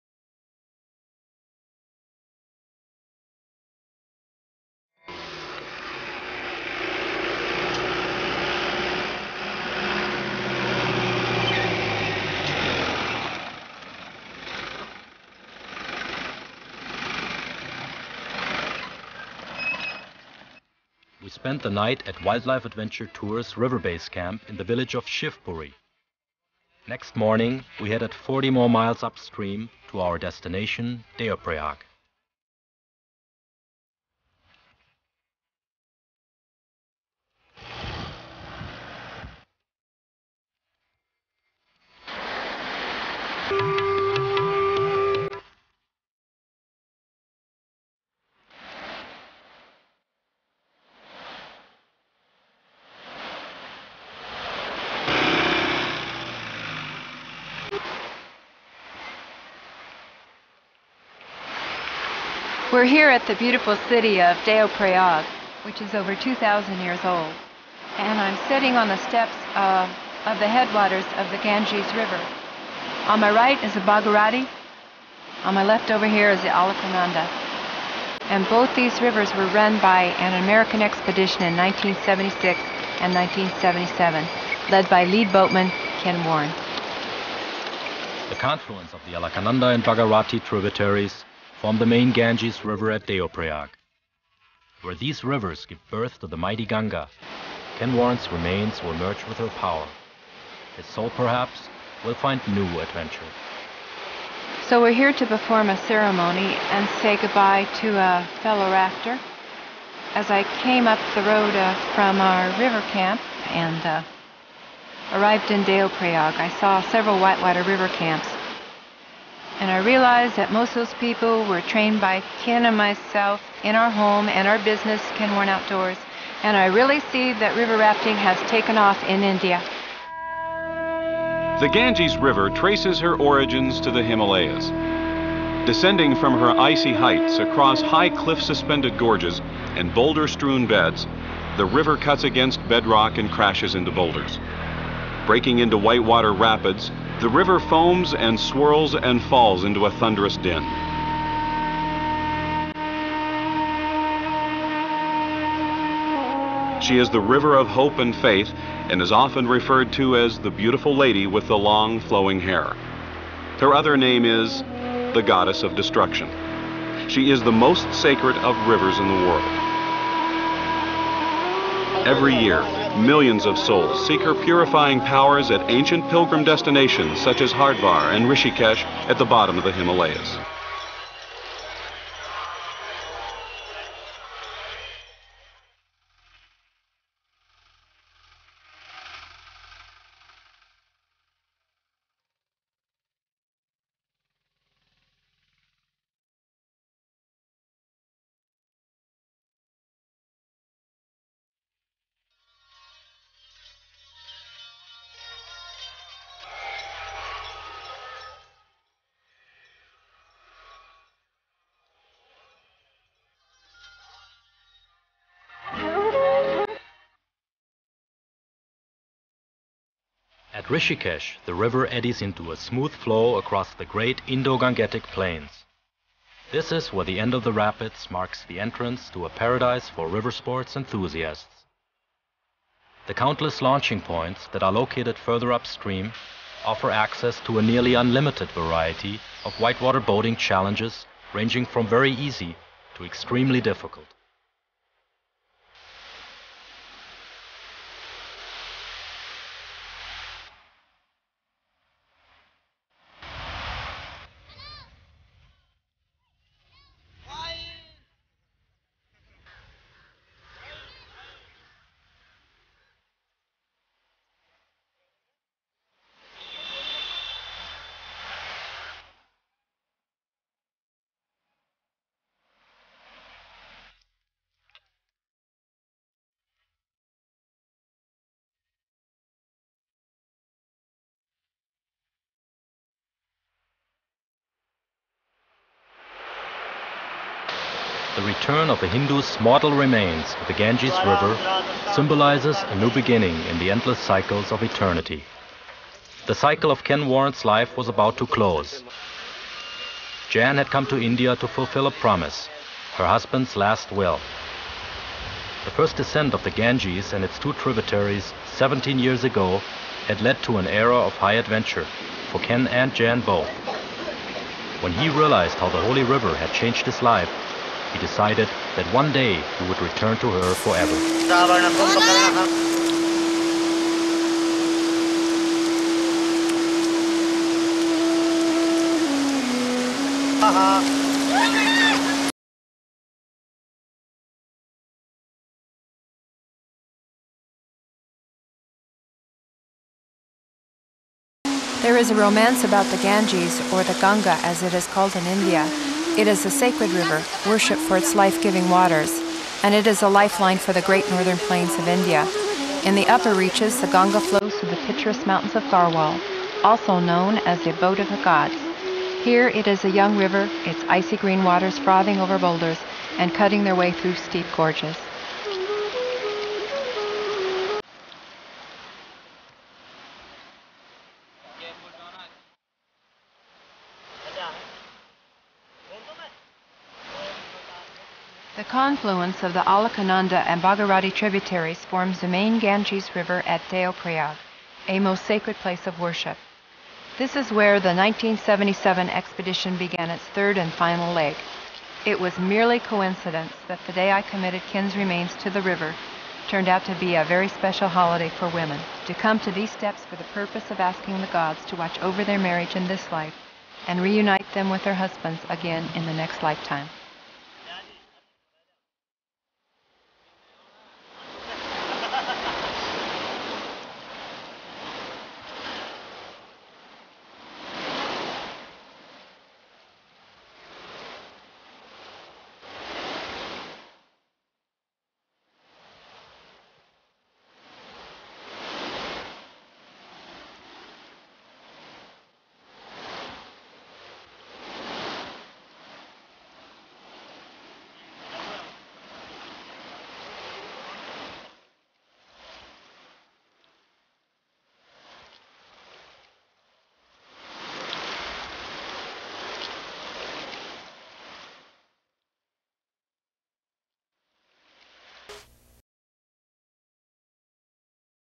Ganga Mayi - River of Return, India 1992 (2)
This video was originally produced as part of a demo. YouTube altered the audio track due to an unresolved copyright matter (concerning some of the music we used).

Final chapter to a true story about life, adventure, loss and triumph...

On a sunny afternoon, in the fall of 1987, I happened upon a copy of Outside, while browsing through the adventure magazine section at Powells bookstore in Portland, Oregon. It was the last day of a weeks visit to Portland, celebrating the conclusion of a long whitewater-rafting season in California.

An article, written by an Outside Magazine staff writer, had caught my attention. After Id finished reading, I rushed to the nearest phone and dialed my friend, the fellow river guide whod invited me to Oregon. Kathy, I said, I just read an article, titled Mutiny on the Yangtze have you heard of Ken Warren? He and his wife run a rafting company, somewhere near Portland. They were involved with a major river exploration in China last year. Yes, Kathy replied, I know about the Warrens and just saw them on the local news, in an interview. A National Geographic photographer died on the expedition and half the team abandoned them, literally mid-stream. Now theyre putting together a new team, to head back and finish what they started.

At first, while paging through the magazine, breathtaking photographs of this adventure peaked my interest. Secondly (as I began reading), I noticed the authors accusatory tone toward the expedition leaders, a rather negative reflection as to how their pursuit of a lifelong dream, caused tragedy to unfold in the middle of nowhere. A significant chapter of my own life was about to begin, as I had the privilege to meet Ken and Jan the very next day. We became good friends and fellow explorers over the years that followed.

Ive always considered the Warrens story characteristic of the culminating difficulties our civilization is experiencing. Increasingly fragmented into religious and socio-economic strife, our inclination to blame (as opposed to collaborate), has once again delivered us at the brink of opportunity. For whom the bell tolls in pursuit of promise, would recognize the storys gist: on small scale and spectacular backdrop, the adventure sets a metaphor as to what happens when we allow fear and uncertainty to cloud our outlook. And the question subsequently arises how can we refrain from jumping ship as were about to enter the unprecedented turbulences, humanity (Expedition Life on Planet Earth), has yet to navigate? A time when each member of the crew is needed, to assume their unique potential on behalf of the whole.

In 2008, Chinese friends of the Warrens took the initiative and honored Ken Warren with the title Father of River Running in China. Jan had attended the ceremony that year and with plans for a similar commemoration in India (closing the cycle), her book Where Dreams and Fear Collide, will be positioned for release.

Ganga Mayi  River of Return is the title of a short no-budget documentary, a few of us put together in 1992. Filmed on SVHS videotape (and supportive materials by various contributors), the doc summarizes Kens life and celebrates its conclusion. Originally produced for memorial/archive and demo purpose only, the piece is rough (so, be prepared)

Ever tried to shoot and host a film at the same time? Cant remember why I did it that way, but, go ahead, laugh all you want about the shorts (thats how we wore them back then :-)